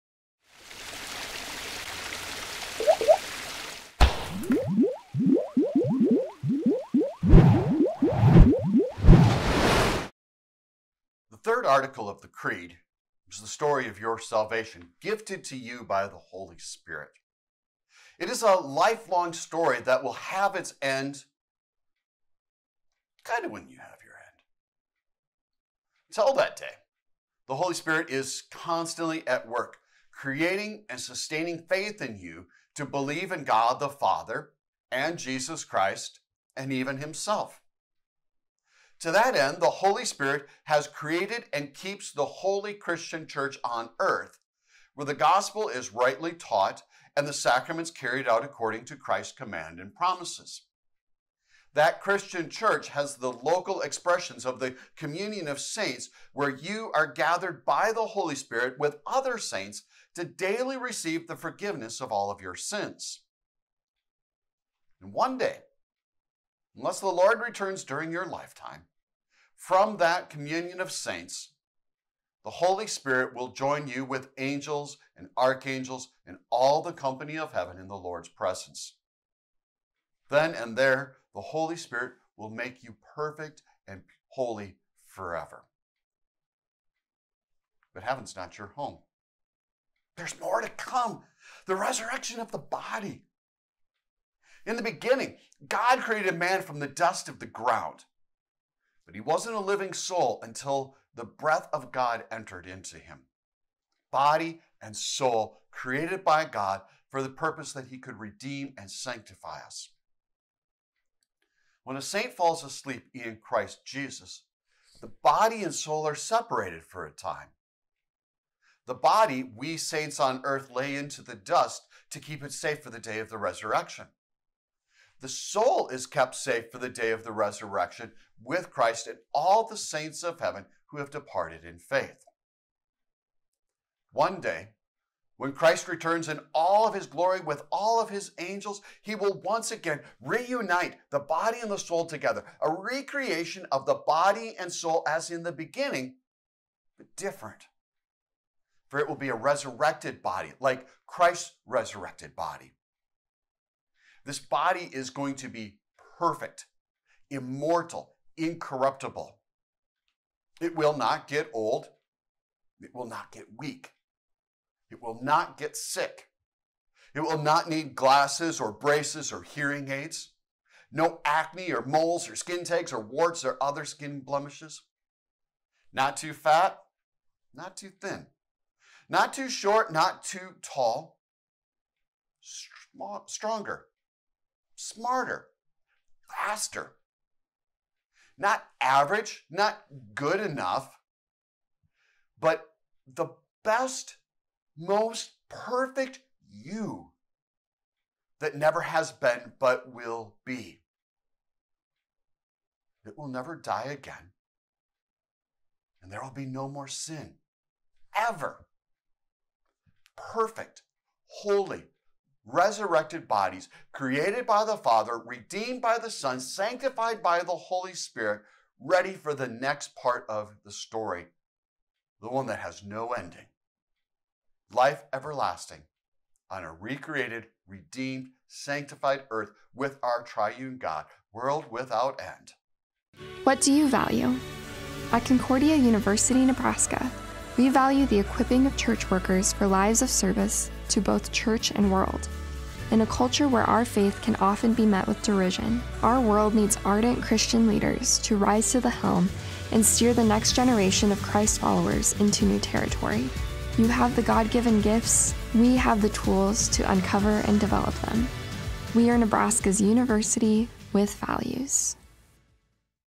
Reuniting Body and Soul
Pastor Voth is back to talk about the reuniting of your body and soul in the resurrection of the saints.

Host: Pastor Daniel Voth is a Pastor of Immanuel Lutheran Church in Grand Forks, ND.